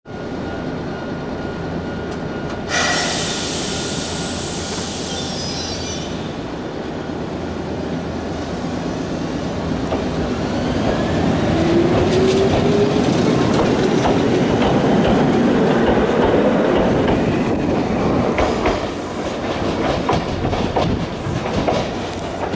一畑電車2100系 出雲大社前駅発車
一畑電車の2100系が出雲大社前駅を発車するシーンです。